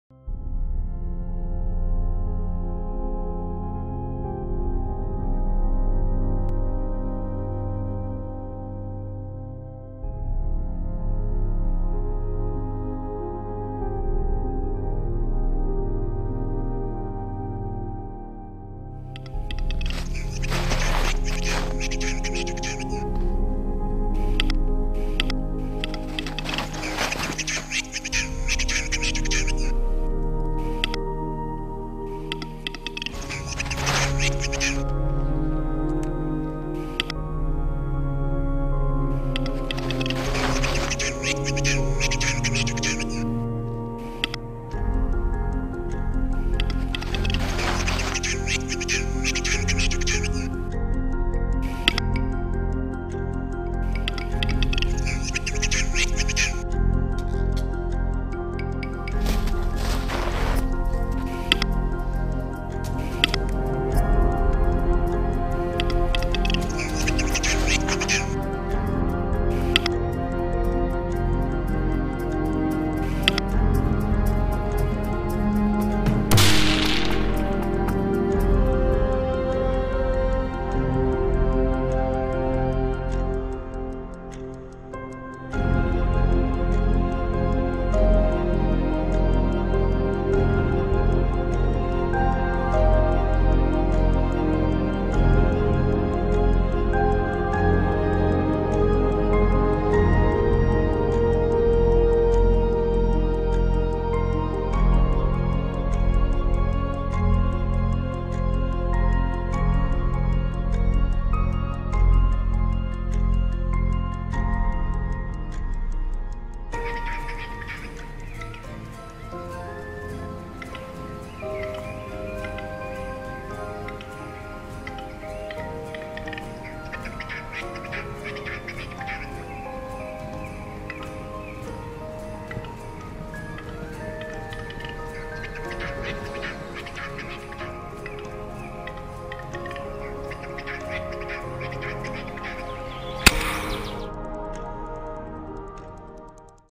Wood Grouse Hunting in Bulgaria
A wonderful video of one of our guests on a wood grouse hunt in Bulgaria. Listen closely to hear this bird's distinct mating call!

If you're interested in trying your hand at this challenging type of hunting, visit our